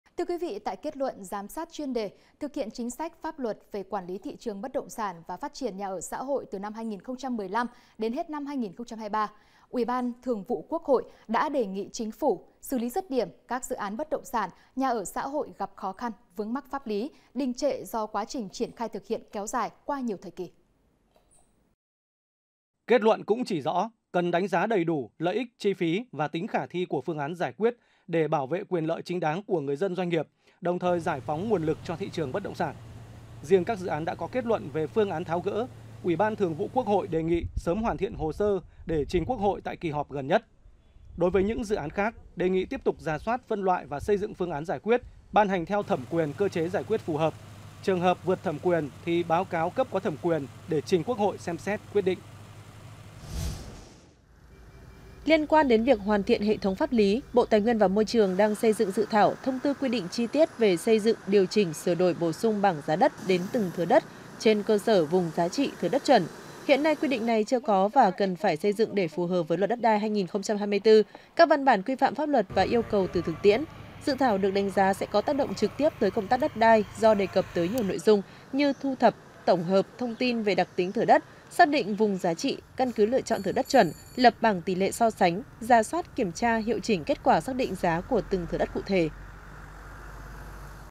Xây dựng bảng giá đất đến từng thửa đất | Tin tức
Liên quan đến việc hoàn thiện hệ thống pháp lý, Bộ Tài nguyên và Môi trường đang xây dựng dự thảo “Thông tư quy định chi tiết về xây dựng, điều chỉnh, sửa đổi, bổ sung bảng giá đất đến từng thửa đất trên cơ sở vùng giá trị, thửa đất chuẩn”. Hiện nay qui định này chưa có và cần phải xây dựng để phù hợp với Luật Đất đai năm 2024, các văn bản quy phạm pháp luật và yêu cầu từ thực tiễn. Dự thảo được đánh giá sẽ có tác động trực tiếp tới công tác đất đai do đề cập tới nhiều nội dung như: Thu thập, tổng hợp thông tin về đặc tính thửa đất; xác định vùng giá trị; căn cứ lựa chọn thửa đất chuẩn; lập bảng tỷ lệ so sánh; rà soát, kiểm tra, hiệu chỉnh kết quả xác định giá của từng thửa đất cụ thể…